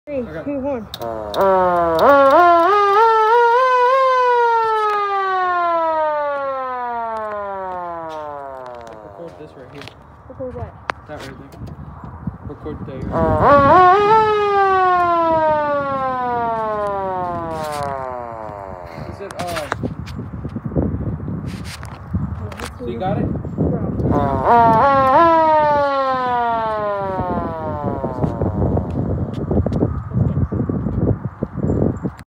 Model A siren growls
AR timer used from Colin (owscrazy1) and my Model A connected.